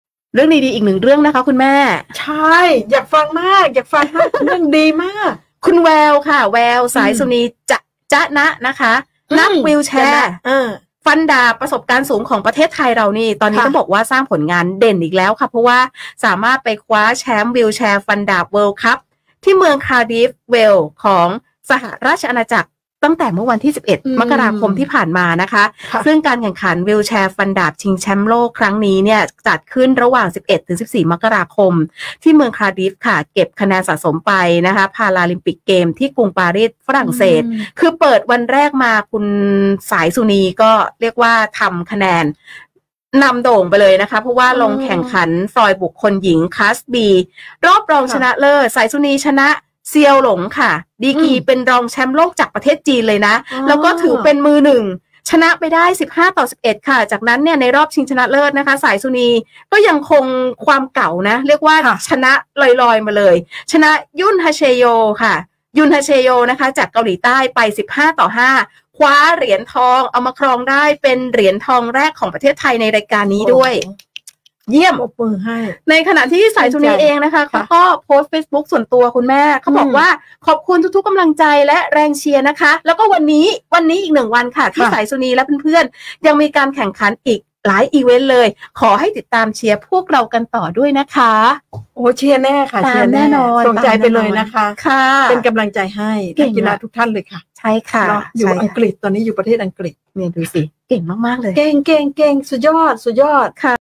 "สายสุนีย์" ตำนานวีลแชร์ฟันดาบไทย ประเดิมแชมป์ฟันดาบเวิลด์คัพ
ไทยผงาดคว้าแชมป์โลก! "สายสุนีย์" ตำนานวีลแชร์ฟันดาบไทย ประเดิมแชมป์ฟันดาบเวิลด์คัพ

12 มกราคม 2567 ผู้สื่อข่าวรายงานว่า “แวว” สายสุนีย์ จ๊ะนะ นักวีลแชร์ฟันดาบประสบการณ์สูงของไทย คว้าแชมป์ “วีลแชร์ฟันดาบ เวิลด์คัพ” ที่เมืองคาร์ดิฟฟ์, เวลส์ ของสหราชอาณาจักร เมื่อ 11 ม.ค. 67 ที่ผ่านมา

วีลแชร์ฟันดาบชิงแชมป์โลก “Wheelchair Fencing 2024 World Cup” ระหว่างวันที่ 11-14 ม.ค. 67 ที่เมืองคาร์ดิฟฟ์ เก็บคะแนนสะสมไปพาราลิมปิกเกมส์ ที่กรุงปารีส ประเทศฝรั่งเศส เปิดวันแรก สายสุนีย์ จ๊ะนะ นักฟันดาบระดับตำนาน ลงแข่งขันฟอยล์บุคคลหญิง คลาสบี

รอบรองชนะเลิศ สายสุนีย์ ชนะ เซียว หรง ดีกรีรองแชมป์โลกจากจีน และเป็นมือ 1 ไปได้ 15-11 จากนั้นในรอบชิงชนะเลิศ สายสุนีย์ ยังยอดเยี่ยม ใช้ความเก๋า ชนะ ยุน ฮเย โช จากเกาหลีใต้ 15-5 คว้าเหรียญทองมาครอง เป็นเหรียญทองแรกของไทยในรายการนี้

ขณะที่สายสุนีย์ ได้โพสต์เฟซบุ๊กระบุว่า “ขอบคุณทุกๆ กำลังใจและแรงเชียร์นะคะ วันนี้ (12 ม.ค.) สายสุนีย์และเพื่อนๆ ยังมีแข่งขันอีกหลายอีเวนต์ ติดตามเชียร์พวกเรากันต่อนะคะ”